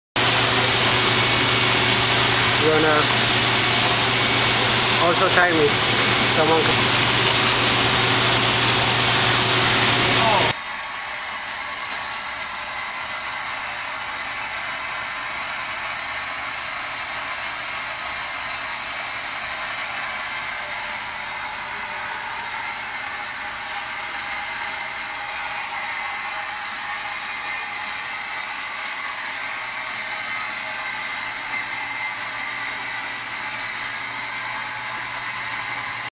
promotional video raw.wmv
Senior Design Project: this is the raw video of the test of our Impact/Wear test machine